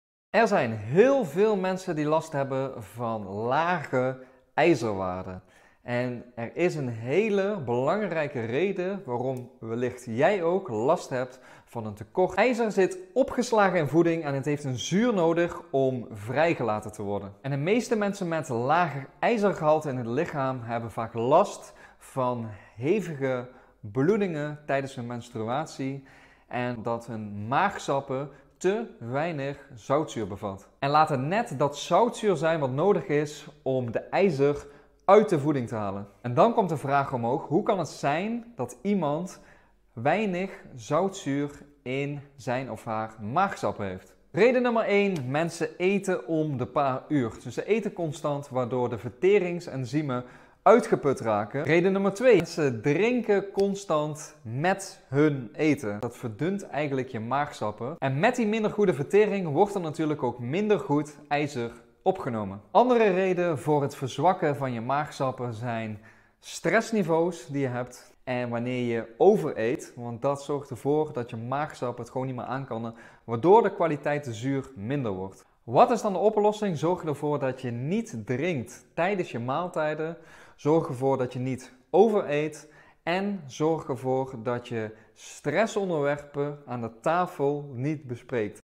Waarom Lage IJzerwaarden
Veel mensen hebben lage ijzerwaarden! Hoe komt dit en wat is er tegen te doen? Sommige aandoeningen, zoals coeliakie of de ziekte van Crohn, kunnen de opname van ijzer in de darmen belemmeren. Andere medische aandoeningen, zoals sikkelcelanemie, kunnen leiden tot chronische afbraak van rode bloedcellen en daardoor tot lage ijzerwaarden. Behandeling en management van de onderliggende ziekte zijn essentieel!

In deze video dus de oorzaken van ijzertekort en hoe je dit kunt gaan verbeteren. Laatste 'pro-tip': Voeg vitamine C-rijke voedingsmiddelen toe aan je dieet, zoals citrusvruchten, paprika's en aardbeien en zeker ascorbinezuur(poeder) omdat vitamine C de opname van ijzer enorm kan verbeteren. Tag vrienden / familie die hier baat bij kunnen hebben.